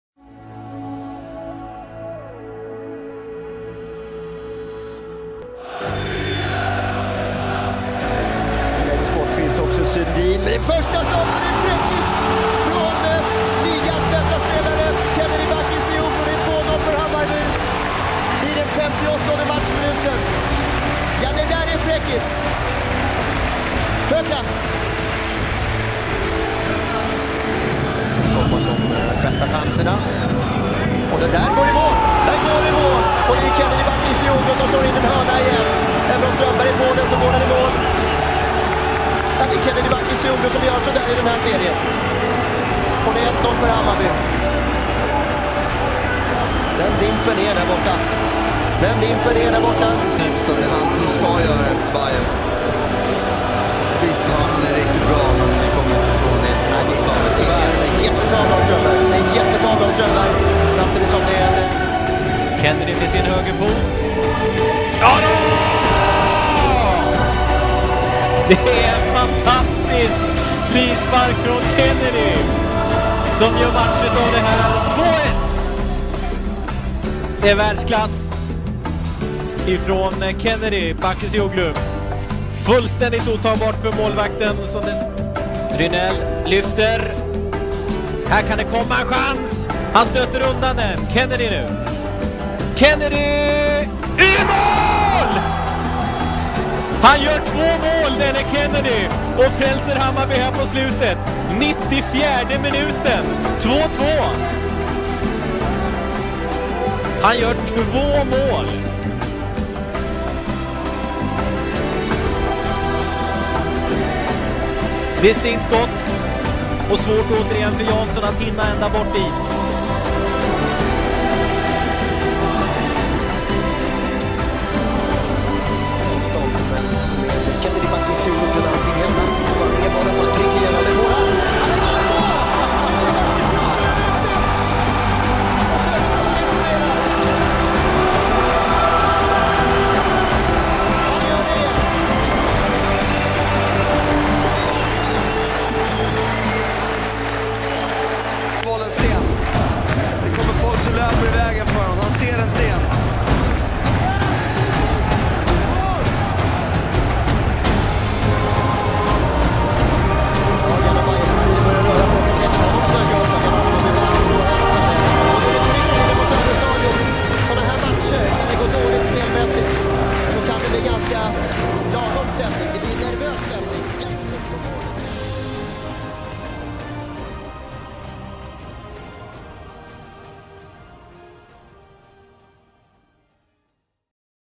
Kennedy Bakircioglu ● Amazing Goals [2013]
Kennedy Bakircioglu amazing goals 2013 in Hammarby IF. Swedens best player when it comes to set pieces..